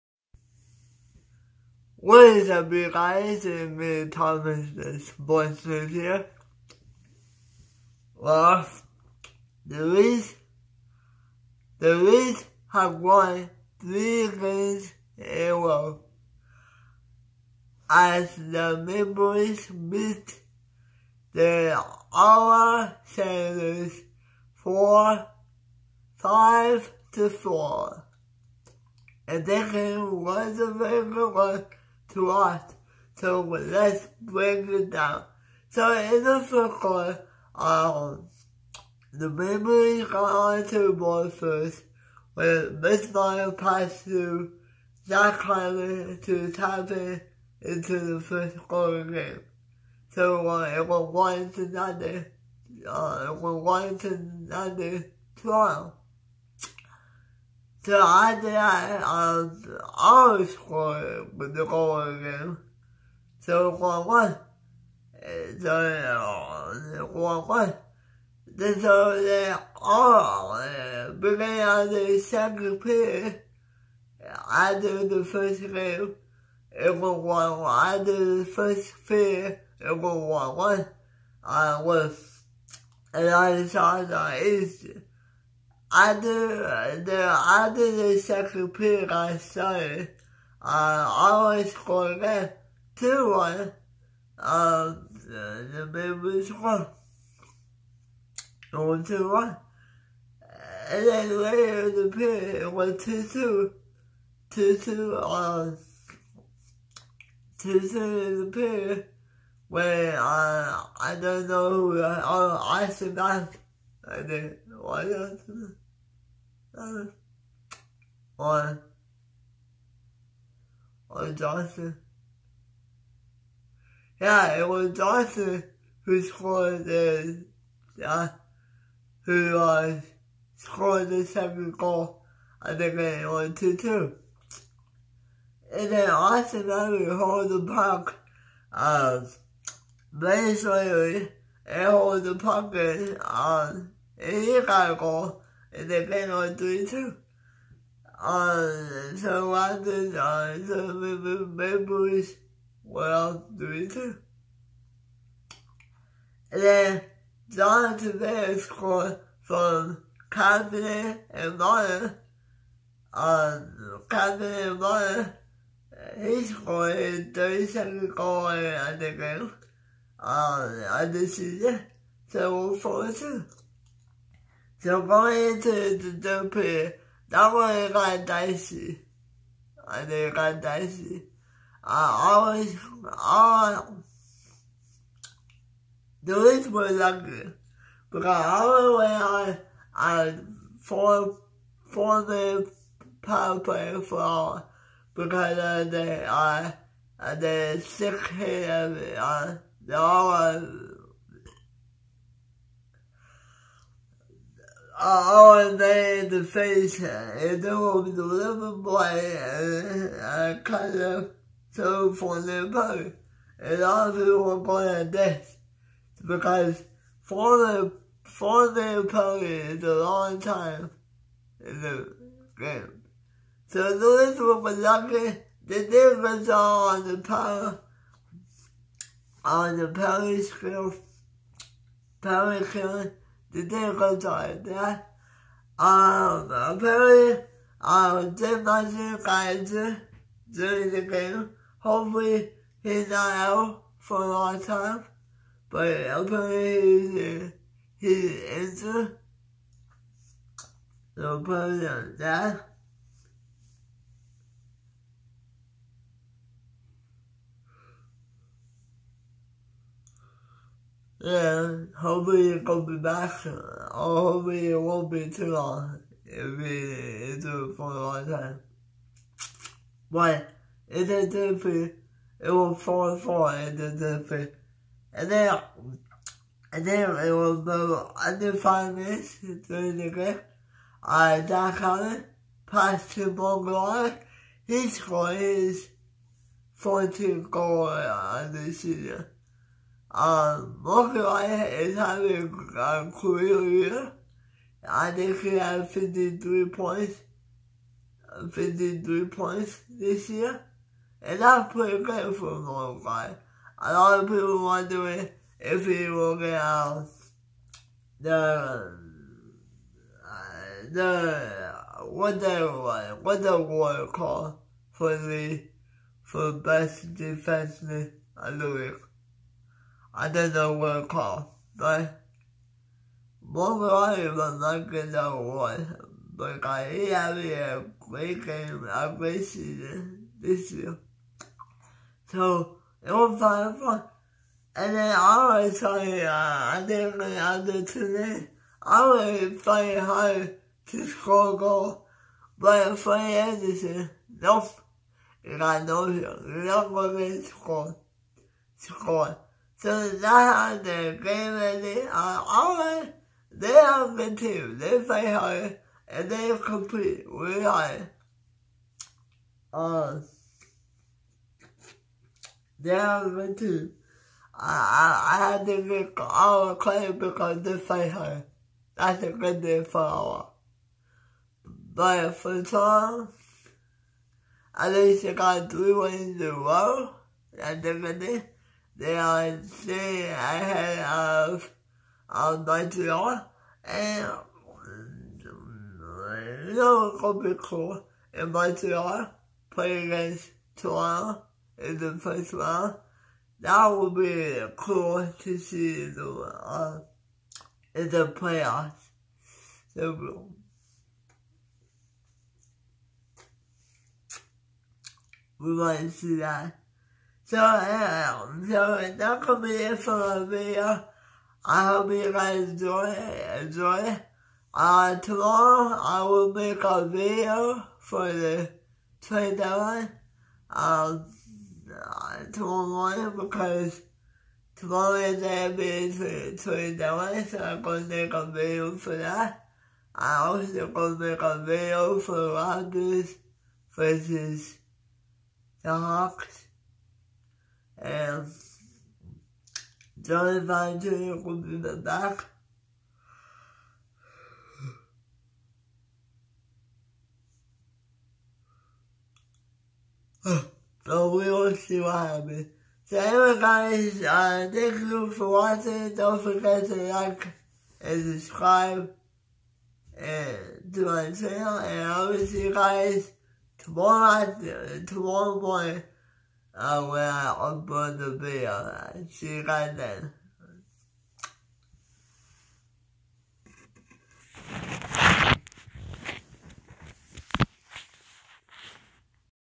Leafs vs Senators Game 53 Postgame Report
The Leafs had now win 3 games in a row. As the Leafs have beat the Senators 5-4. Marner, Hyman and Tavares had the multiple points in the game against the Senators. The Leafs will play against Canadiens on Saturday night.

Follow me on Twitchtv gamer5794